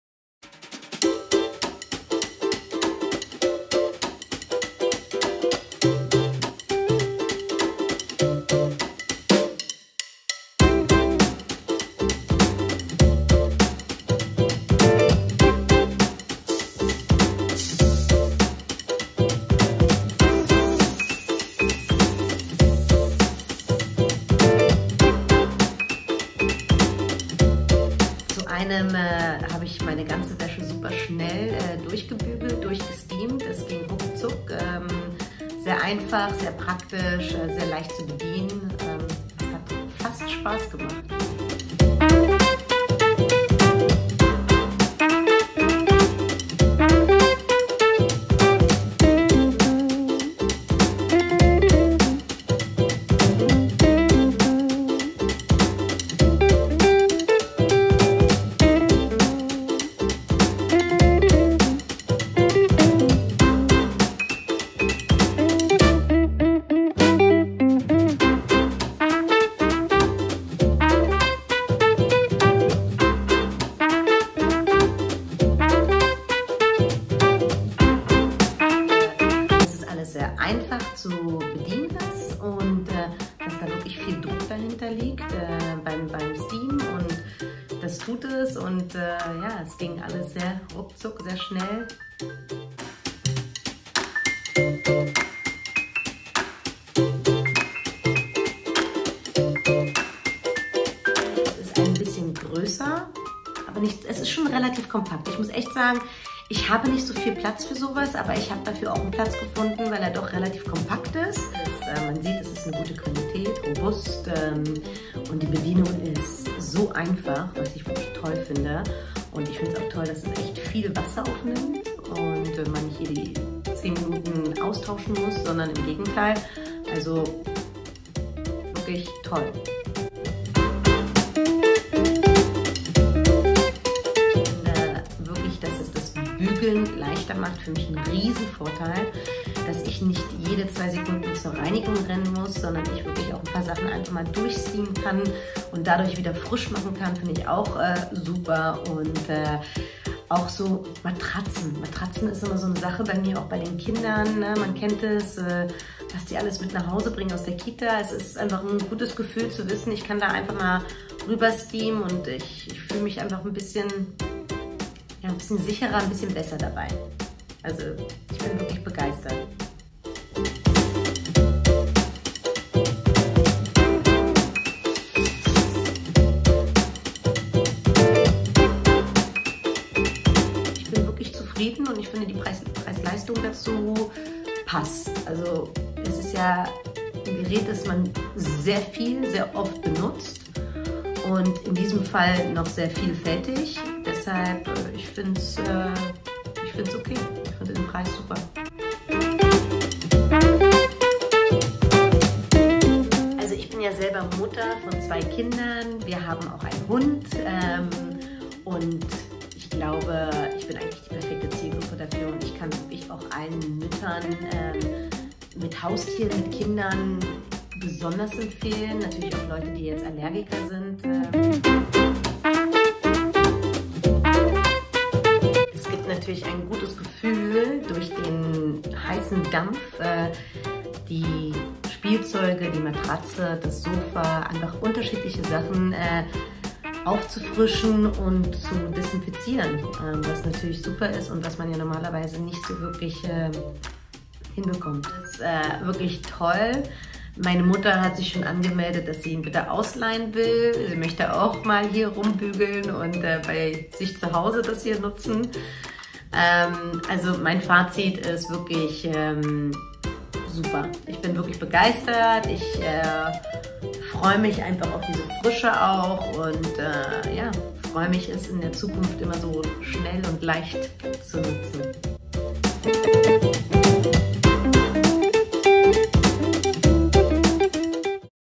mother.NOW testet den Tefal CUBE - check.NOW
Wir testen den Tefal Cube - Steamer, Bügeleisen und Sanitizer in Einem. Schaut selbst!